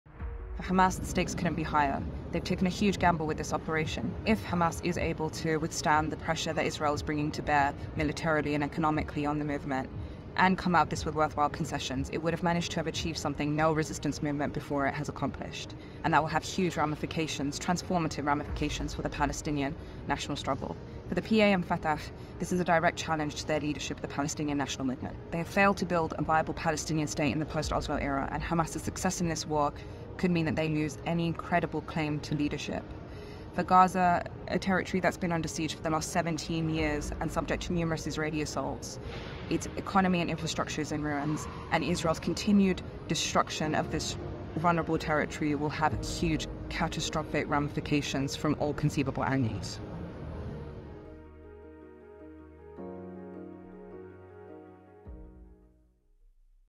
A 360-degree take on Israel-Palestine: Palestine
As the conflict between Hamas and Israel escalates, neighbouring countries and the region as a whole face difficult political challenges, a potentially catastrophic humanitarian crisis, and spreading instability. In these short videos, our expert analysts for Israel, Palestine, Lebanon, Egypt, Iran, the Gulf Arab states, Syria, Iraq and Turkiye are explaining how these countries are responding to the rapidly evolving conflict.